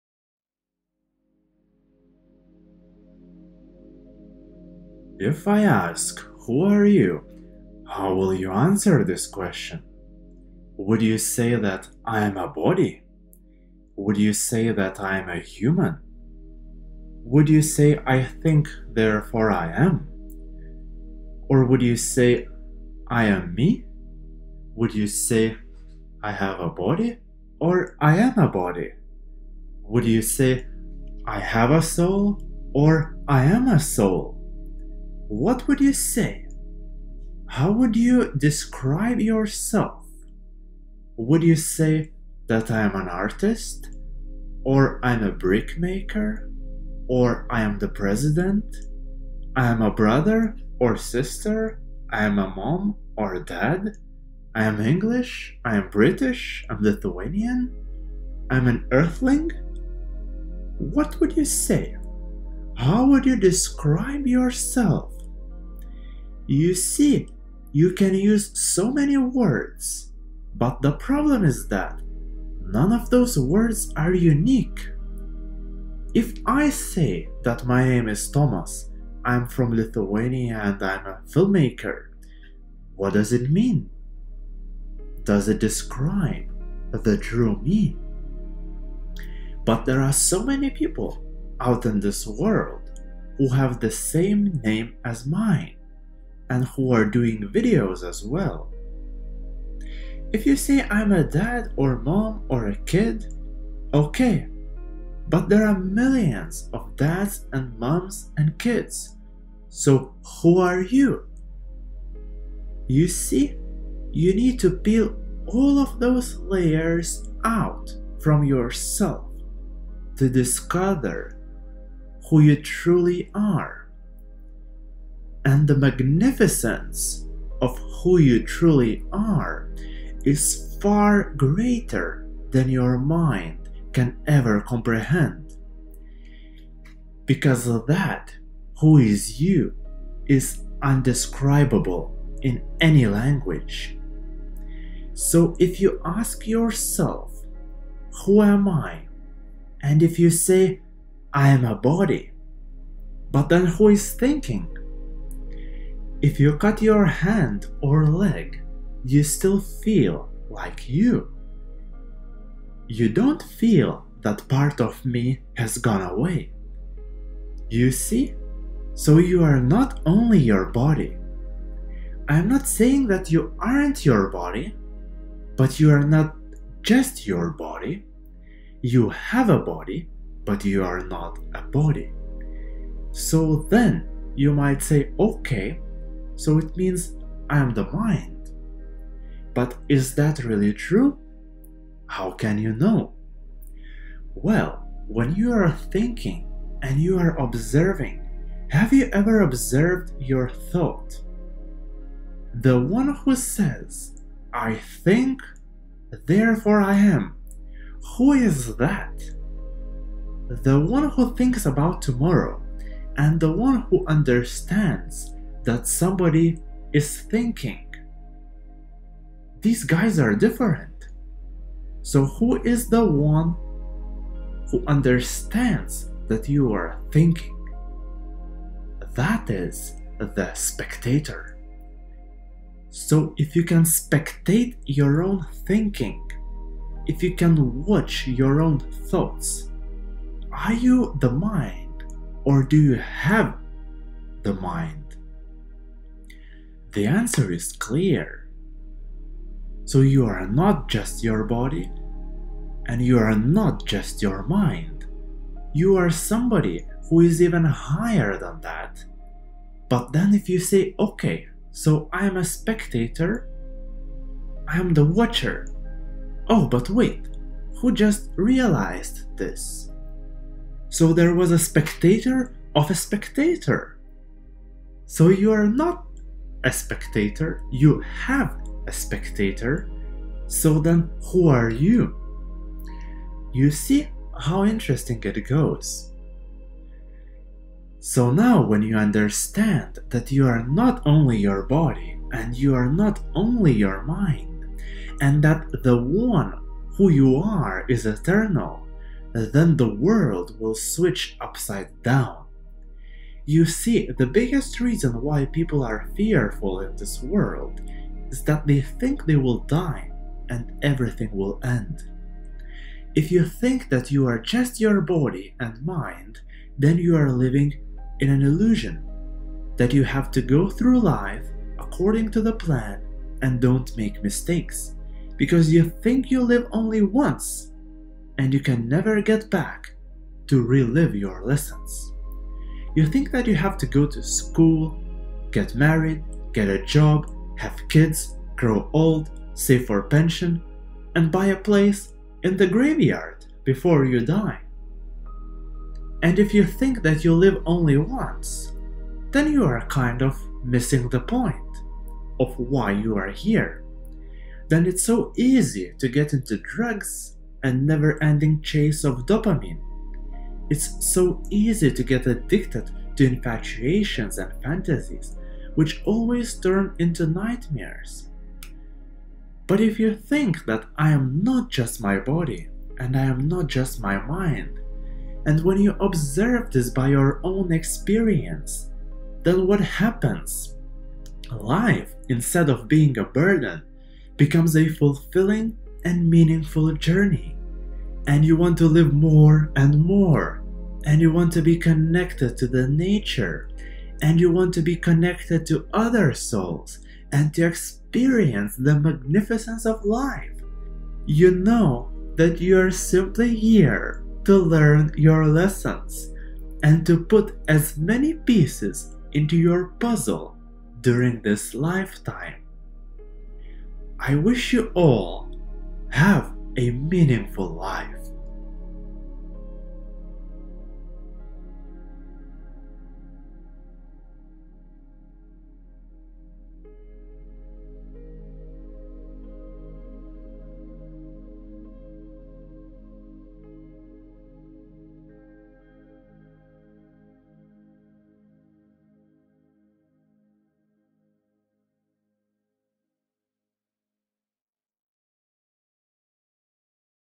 Who are you? Artistic journey into self discovery
If i ask who are you

how will you answer this question

would you say that

i'm a body

would you say that i'm a human

would you say i think therefore i am

or would you say i am me

would you say i have a body or i am a body

would you say i have a soul or i am a soul

what would you say how would you

describe yourself would you

say that i'm an artist

or i'm a brick maker

or i am the president

i'm a brother or sister i'm a mom

i'm english i'm british i'm lithuanian

i'm an earthling

describe yourself

you see you can use so many words but

the problem is

that none of those words are unique

if i say that my name is thomas

i am from lithuania and

i'm a filmmaker

does it describe the true me?

but there are

so many people out in this world

who have the same name as me who are

doing videos as well

if you say i'm a dad or mom or a kid

okay but there are millions of dads and moms and kids - so who are you?

you see you need to

peel all of those layers out

from yourself to discover who you truly are

and the magnificence of who you truly are

is far greater than your mind can ever comprehend

because that

who is you is undescribable

in any language

So if you ask yourself

who am i and if you say: i am a body

but then who is thinking?

if you cut your hand or leg - you still feel like you

i don't feel that part of me has gone away

you see

so you are not only your body

i'm not saying that you aren't your body

but you're not just your body

you have a body - you are not a body

so then you might say okay so i am the mind

but is that really true - how can you know?

well when you are thinking

and you are observing

have you ever observed your thought

the one who says i think therefore i am

who is that

the one who thinks about tomorrow

and the one who understands that

somebody is thinking - who is that?

that is the spectator

so if you can spectate your own thinking

if you can watch your own thinking

are you the mind or do you have a mind

the answer is clear

so you are not just your body and your mind

you are somebody who is even higher than that

but then if you say okay so i'm a

spectator i'm the watcher

oh but wait - who just realized this?

so there was a spectator of a spectator

so you are not a spectator -

you have a spectator

so then who are you?

you see how interesting it goes

so now when you understand that you are

not only your body and you are not only your mind

and that the one who you are is eternal

then the world will switch upside down

you see - the biggest reason why people are fearful in this world

is that they think they will die

and everything will end

if you think that you are just your body and mind

then you are living in an illusion

that you have to go through life according to the plan and don't make mistakes

because you think you live only once and you can never get back to relive your lessons

you think that you have to go to school, get married, get a job, have kids, grow old, save for pension and buy a place in the graveyard before you die

and if you think that you live only once

then you are kind of missing the point

of why you're here

then it's so easy

to get into drugs and never-ending chase of dopamine

it's so easy to get addicted to

infatuations and fantasies which always turn into nightmares

but if you think that i am

not just my body i am not just my mind

and when you observe this by your own experience

then what happens

life, instead of being a burden

becomes a fulfilling and meaningful journey

and you want to live more and more

you want to be connected to the nature

you want to be connected to other souls

and to experience the magnificence of life

you know you're simply here to learn your lessons

and to put as many pieces into your puzzle during this lifetime

have a meaningful life